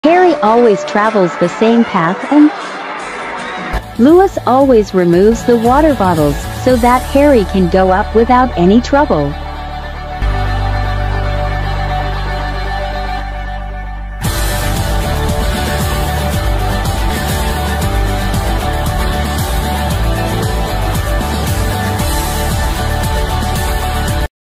louis always clearing the water bottles off the stage for harry so he doesn’t fall 🥺
if this isn’t husband behavior what is?